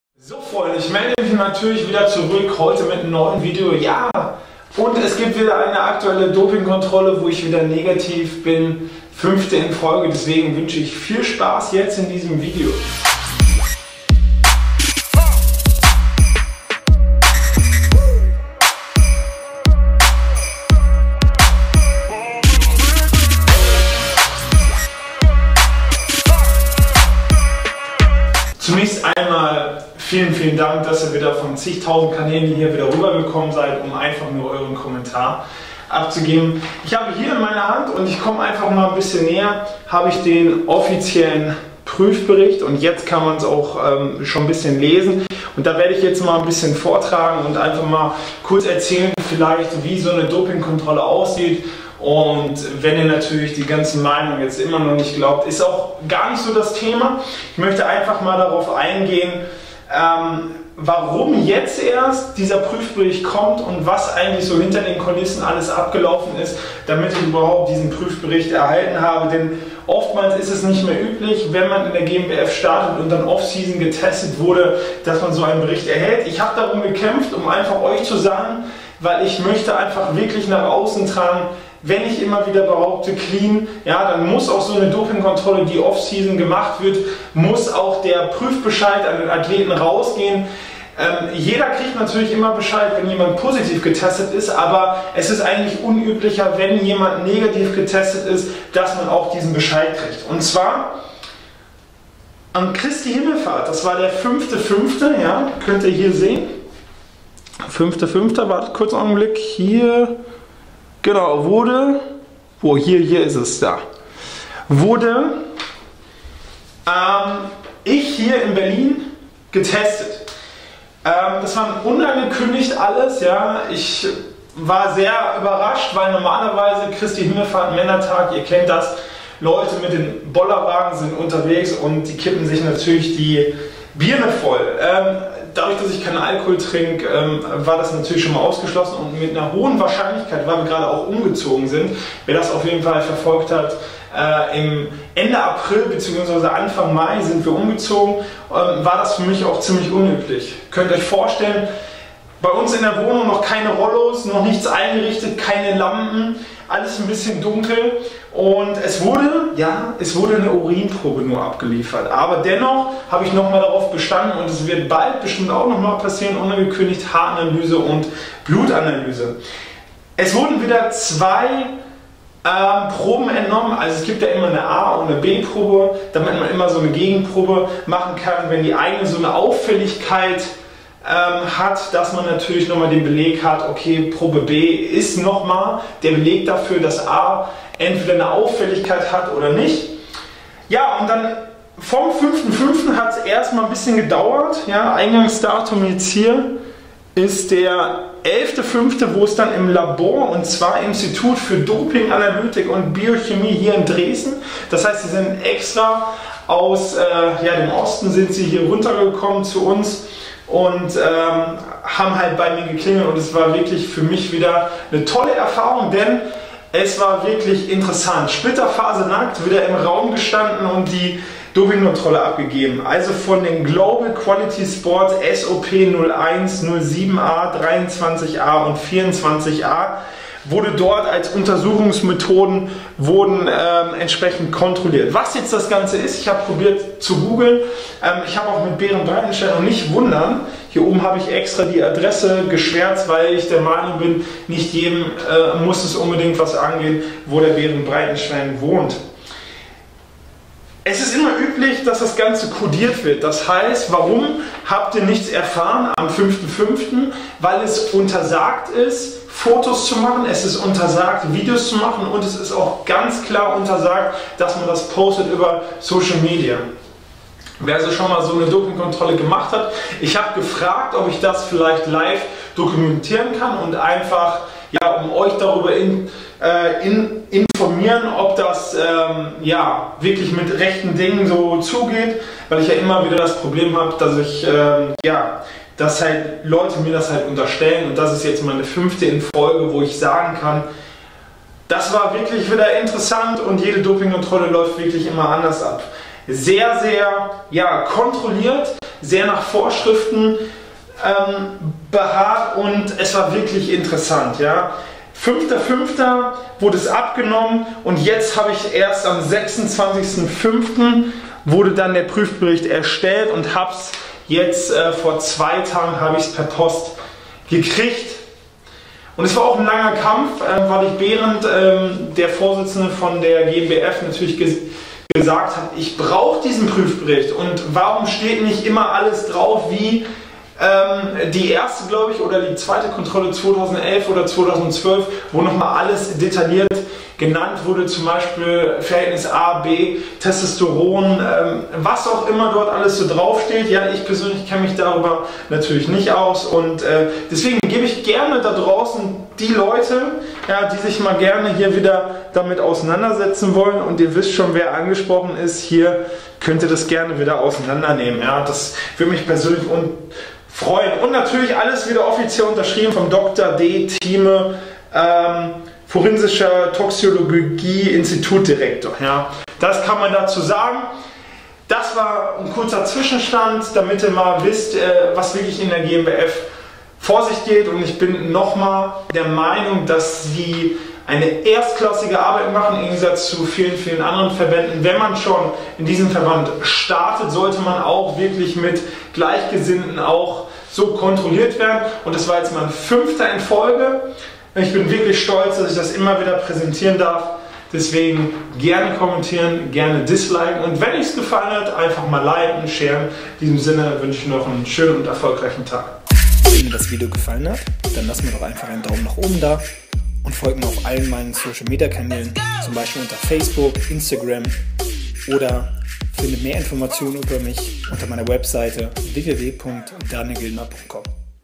Der Dopingkontrolleur stand vor der Tür - Das Ergebnis - Bin ich nicht mehr clean?
Viel Spaß beim Haten!

Powered by BARBOZA™

► SNAPCHAT! ► gildnerofficial 👻
► KIK! ► dgildnerofficial